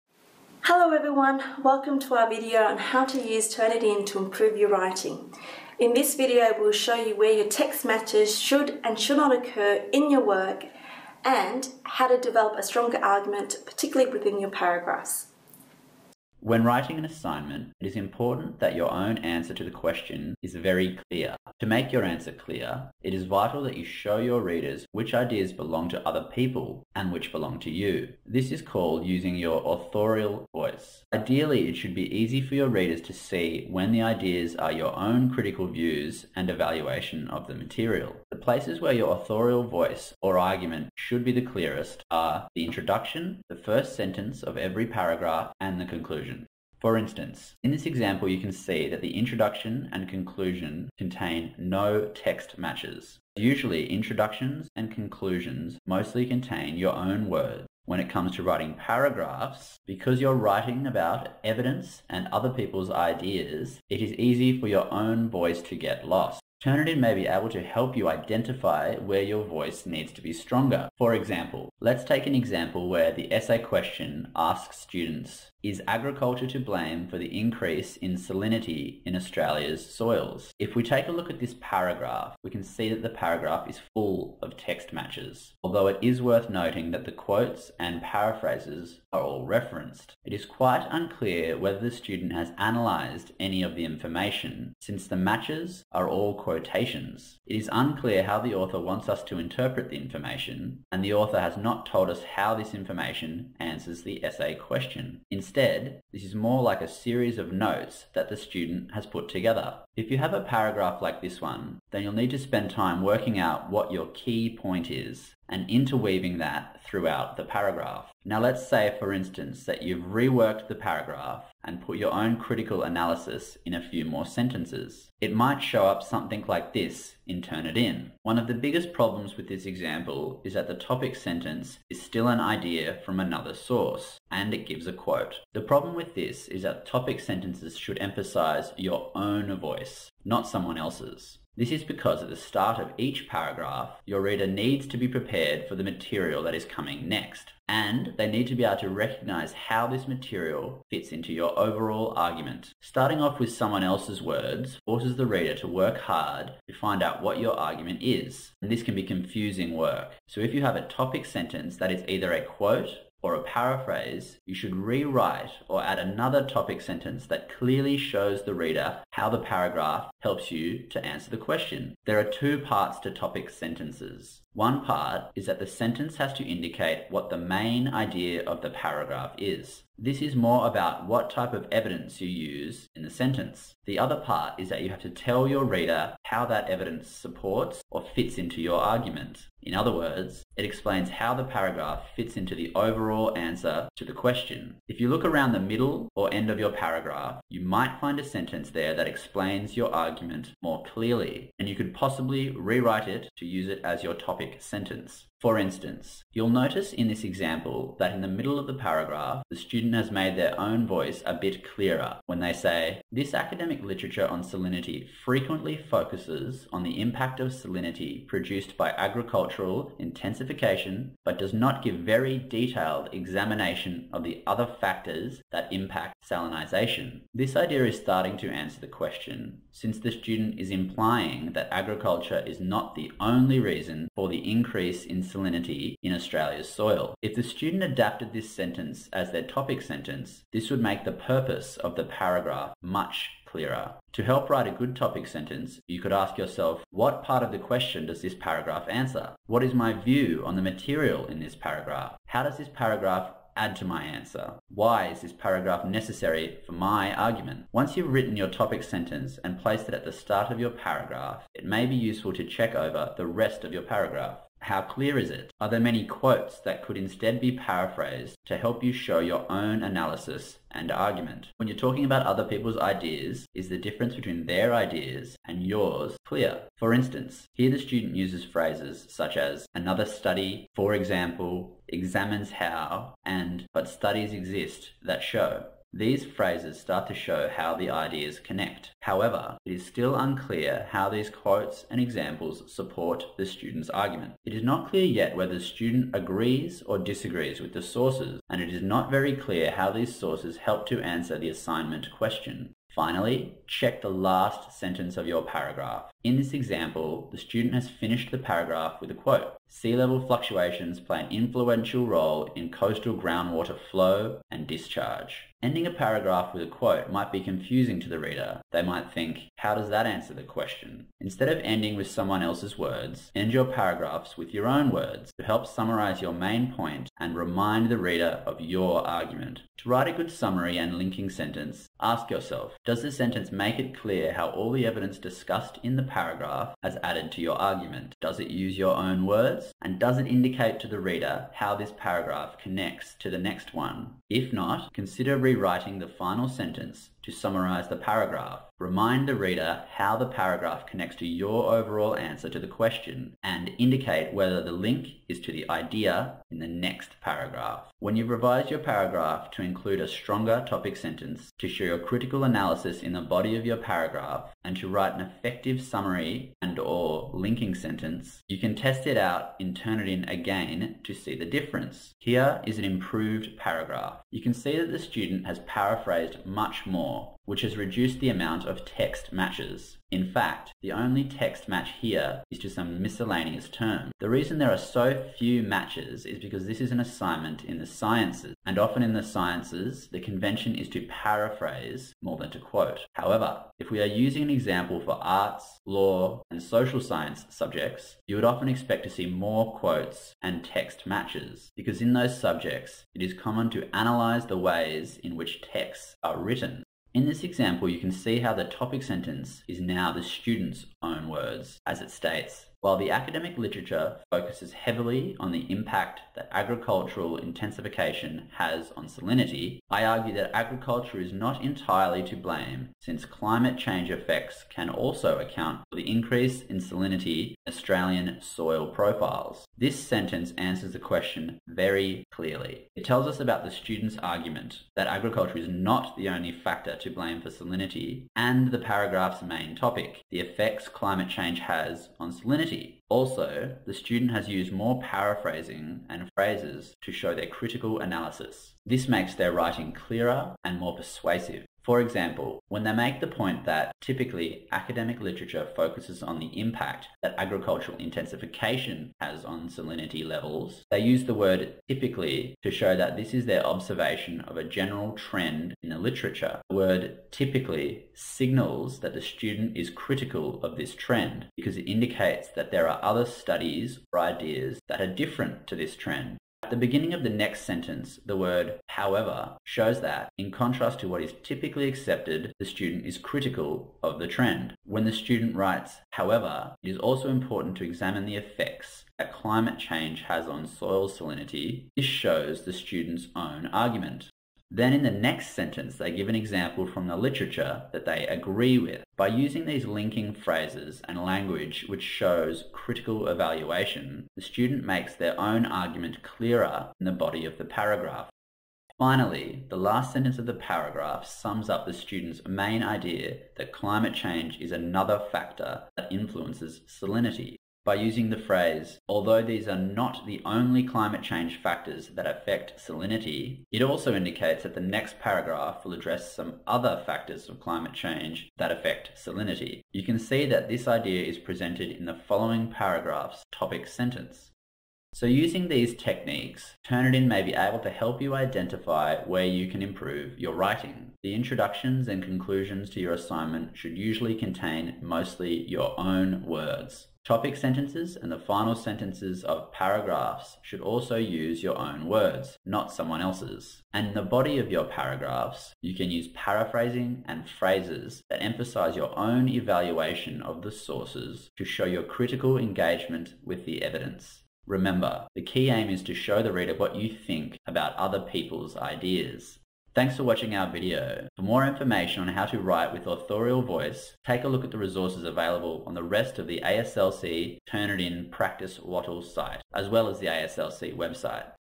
How to improve your writing
This instructional video is used within the ASCL Turnitin Practice site. It demonstrates how to open and interpret your originality report of text matches generated by Turnitin, after having submitted a university assignment to a Wattle (Moodle) drop-box.

The main ANU Academic Skills & Learning Website can be accessed

--- Credits

'How to open and interpret your originality report'
Produced by the Academic Skills & Learning Centre and ANU Online

Script: Viv Silvey
Audio narrator: Conor Owens-Walton
Screen-capture & visuals: Stephanie Kizimchuk
Live footage: Thuy Do & Katie Freund
Audio-visual recording, editing, & video production: Stephanie Kizimchuk
Initial visual advisor: Janene Harman

Our thanks to the ANU students who provided feedback during the production of this video.
This video was funded through an ANU Vice-Chancellor’s Teaching Enhancement Grant: June 2016.

--- License ---

Do share and use but please always acknowledge the original creators.
Category
Education
License